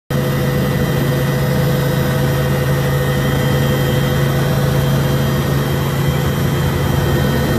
1968 BELL UH-1E For Sale
Transwest Seat mod pilot and copilot seats, New bubble door window and gauges, Can deliver to lower 48 for cost